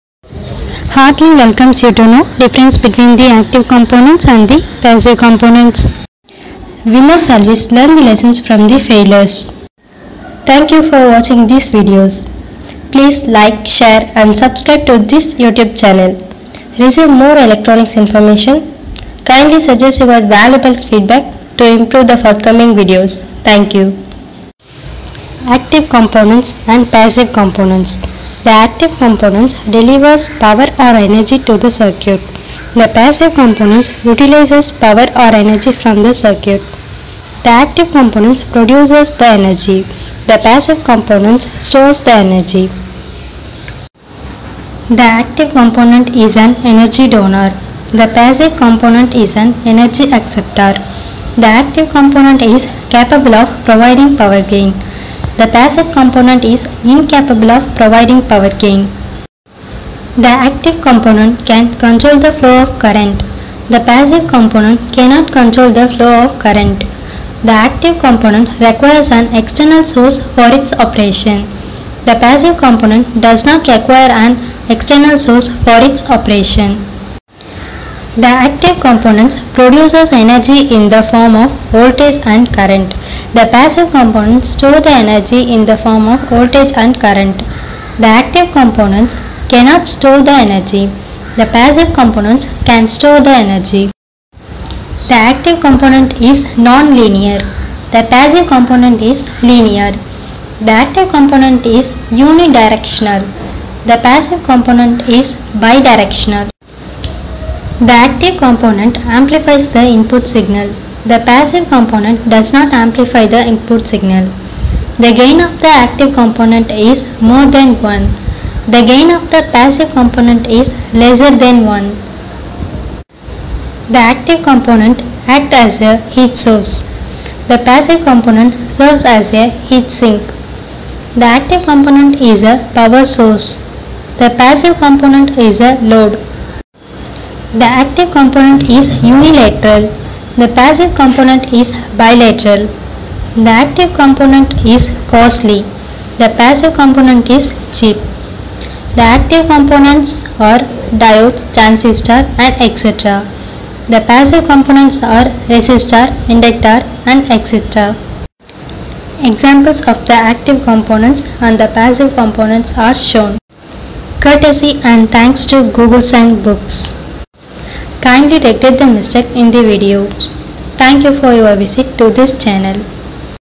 Comparison between active component and passive component.
The video clearly explains differences between
the active and the passive components.
It has the simple explanation with clear notes.
They are easy to understand and notes are
useful.
It helps to attend the competitive examination and face the interview with courage and hope.